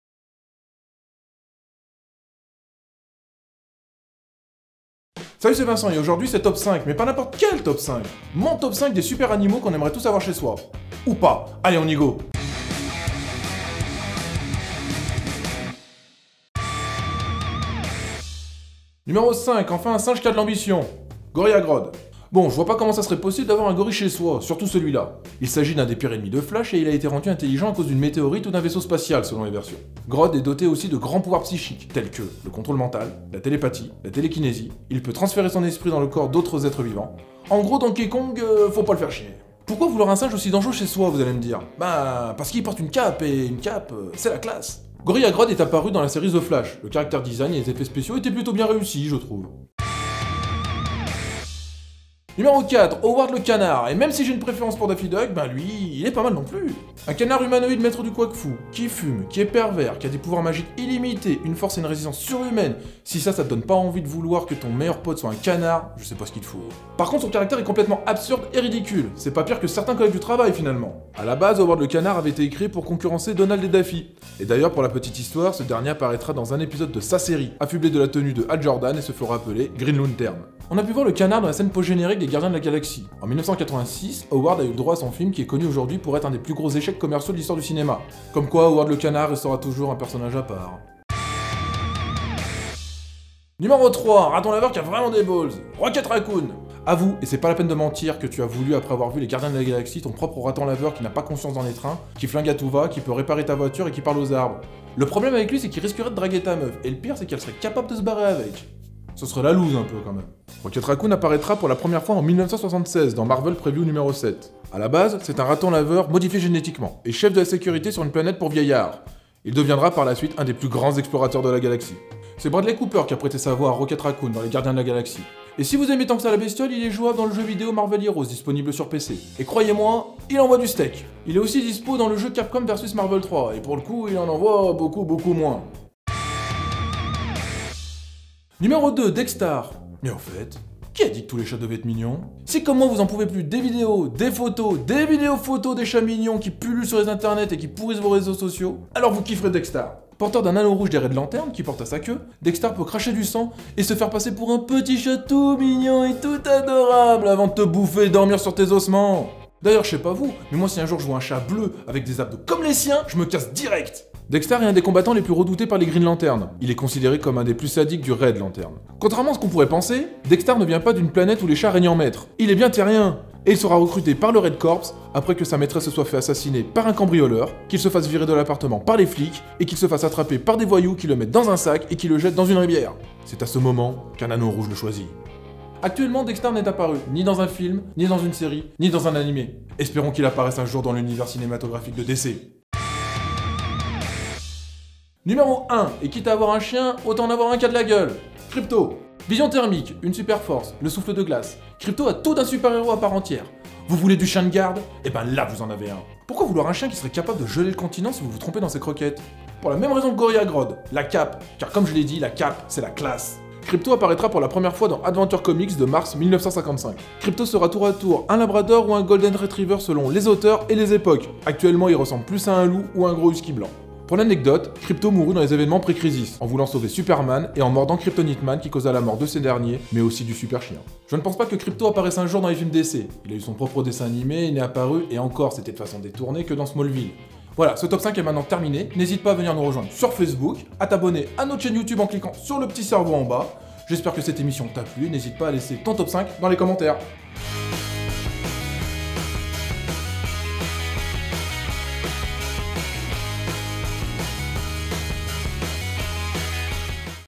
Comics Universe TOP 5 Les Plus Cool Supers Animaux Dans Les Comics
Les animaux aussi portent des capes dans les comics !